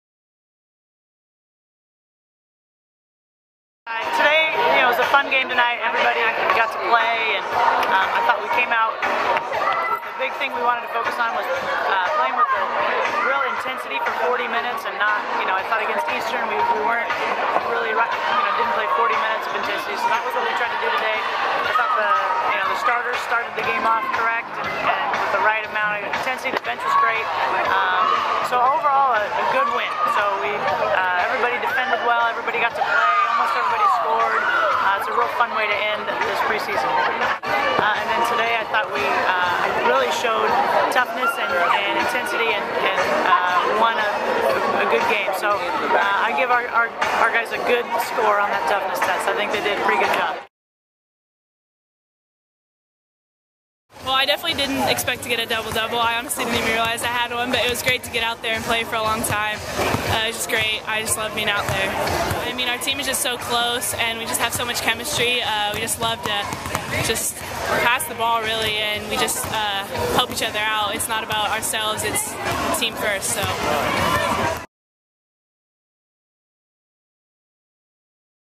WBB: Pacific 95, Air Force 59 Highlights
Highlights and Interviews with head coach Lynne Roberts and freshman forward Kendall Kenyon after the Tigers' 95-59 win over Air Force